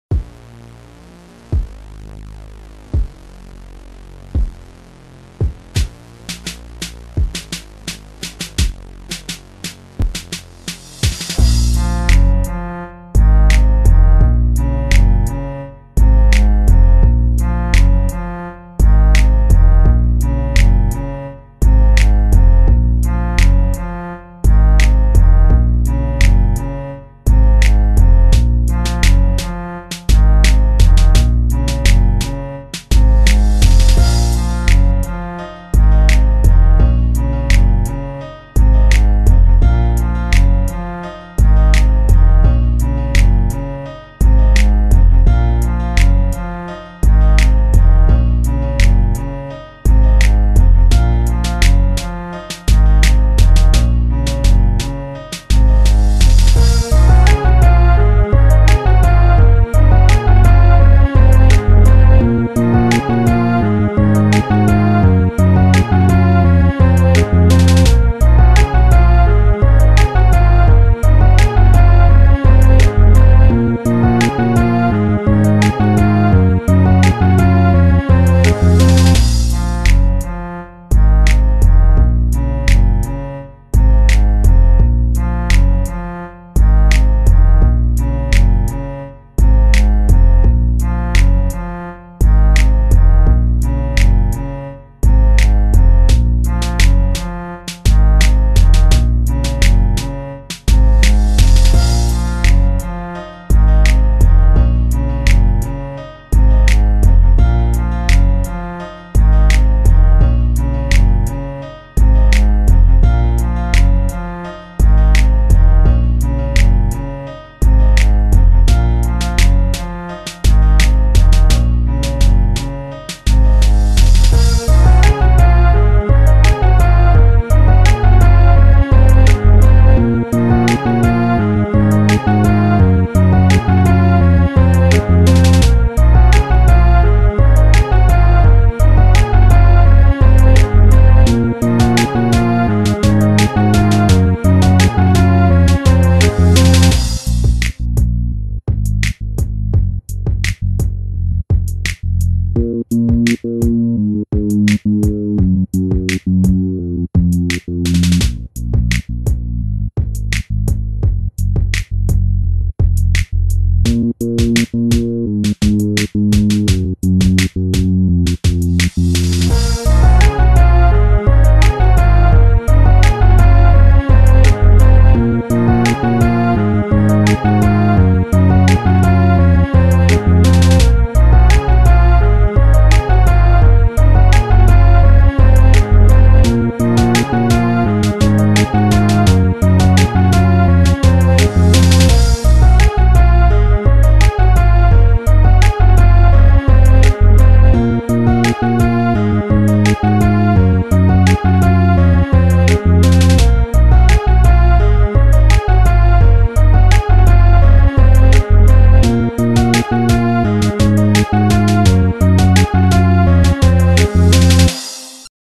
hot HipHop instrumental in fl studio 9
hip hop intrumental
 with random pics of me and other Dumb stuff] so listen &enjoy
 you can also purchase this beat.. hit me in my inbox